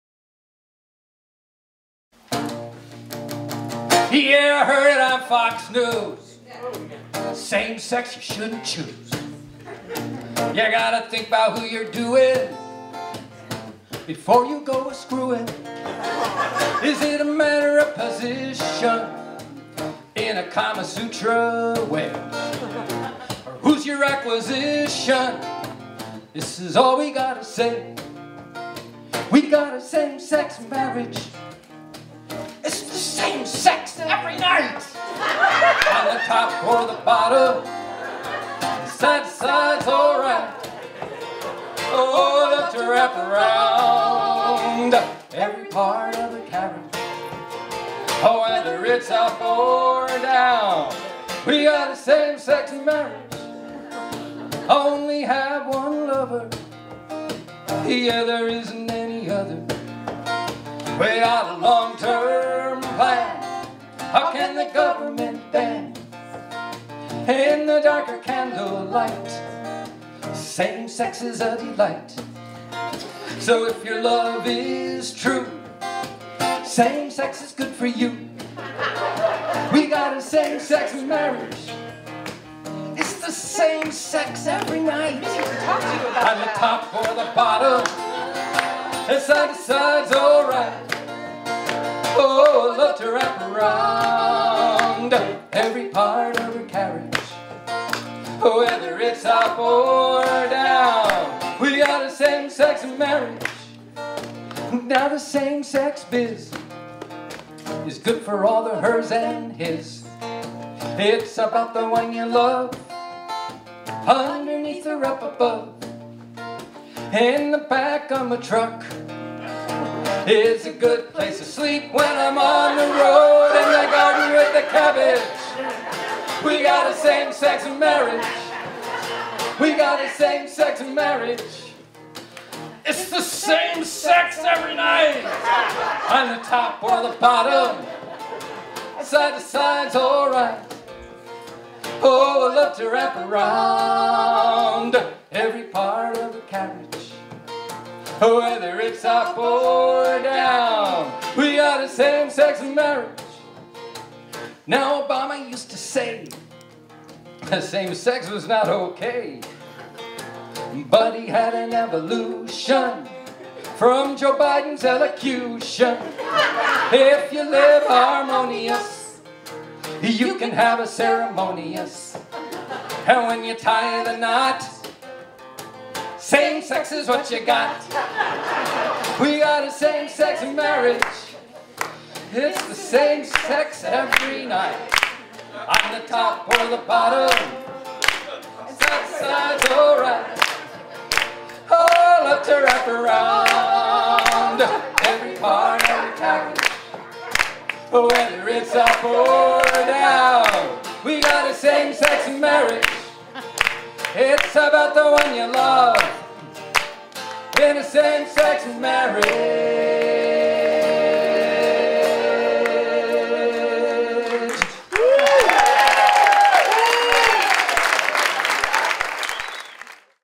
Tom Neilson and Lynn Waldron with Ben Grosscup
Video from Round Robin at People's Music Network 2015 Winter Gathering in Greenfield, MA